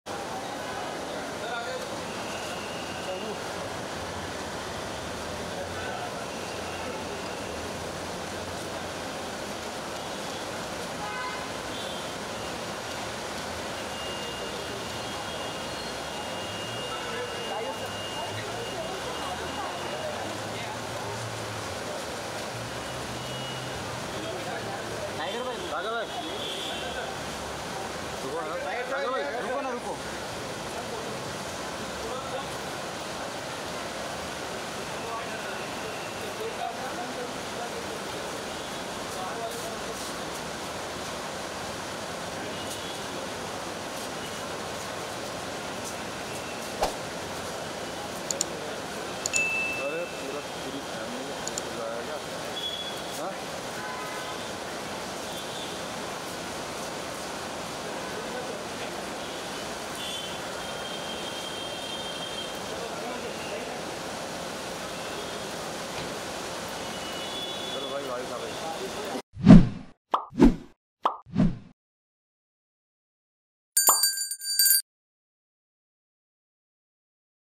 Tiger Shroff Shows Off His CUTS,Body Transformation For Upcoming Film Ganapath & Bade Miyan Chote
Tiger Shroff Show Off His CUTS,Body Transformation For Upcoming Movie Ganapath & Bade Miyan Chote Miyan | Access Bollywood

Access Bollywood  :-